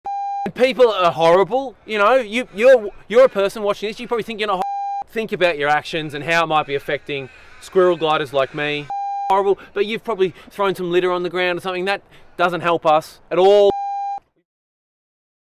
David Quirk is the Squirrel Glider Highlight 3
Ash London interviews the Squirrel Glider (David Quirk) about what it's like to be on the edge of extinction.